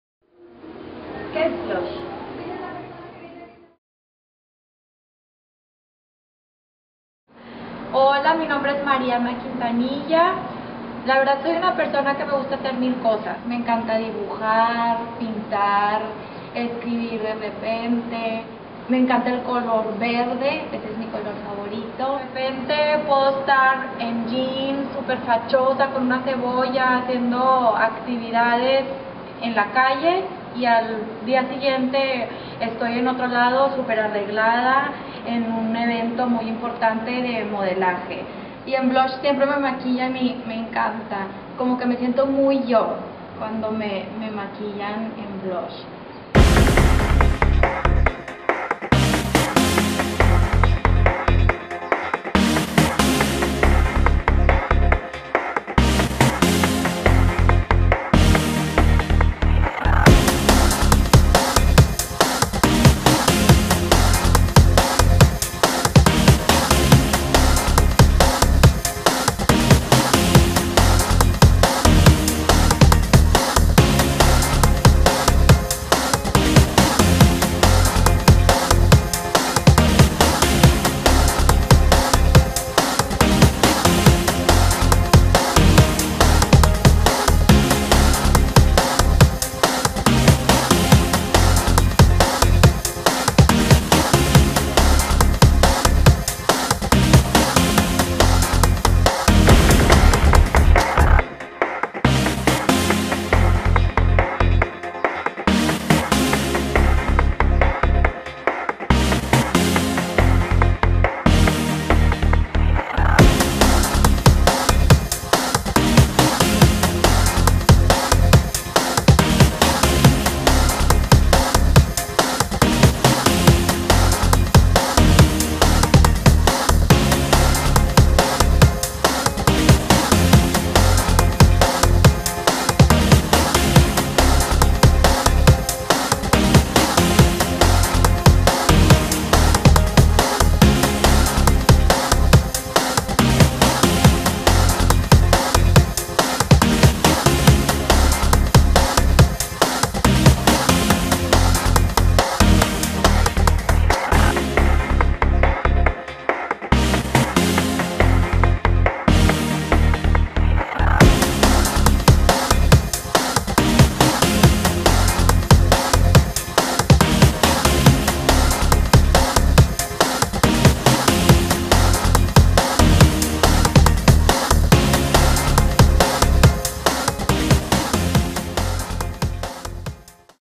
que es blush?
En blush nos preocupamos por tus gustos, tu personalidad, aquello que te apasiona y todo eso lo traducimos a nuestro arte para que tu look sea solo tuyo. Nuestro objetivo es reflejar tu belleza interna.

ombre highlights, signature haircut, waves and makeup by blush
fotografia y video por Barbara Casso
modelo Mariana Quintanilla